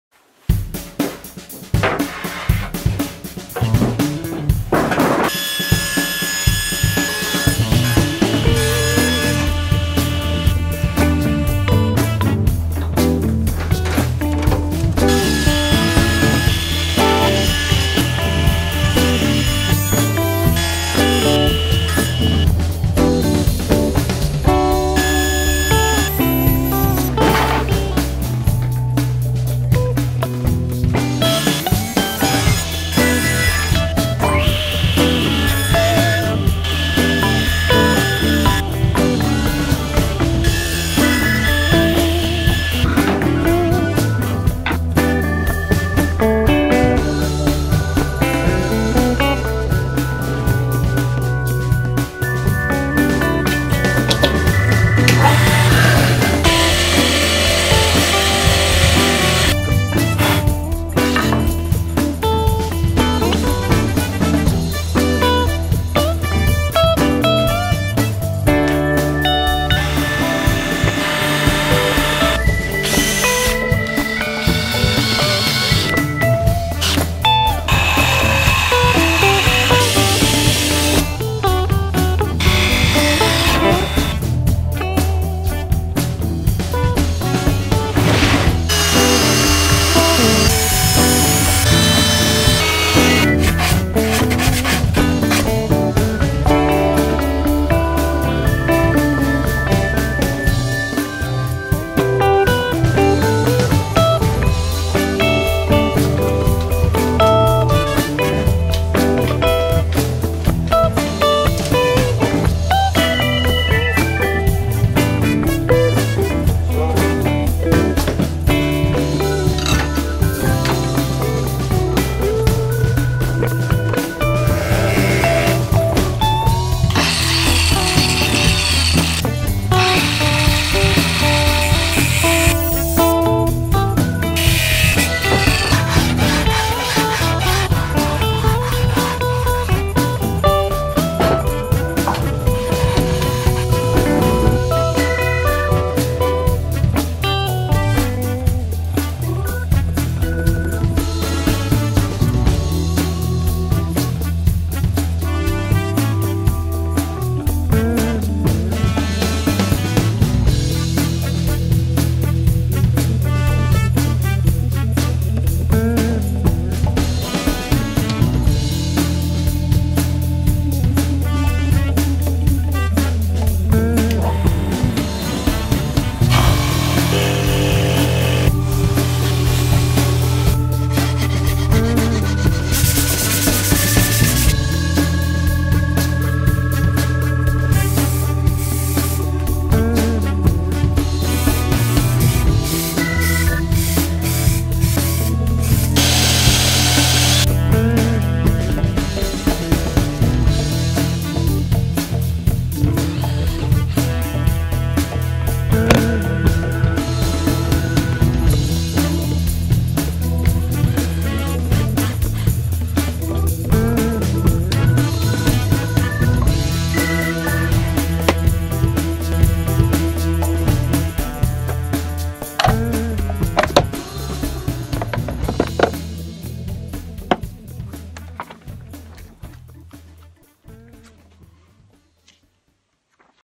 Making Simon`s night table
Simon is making a night table (bedside table in Norwegian)

Ask if anything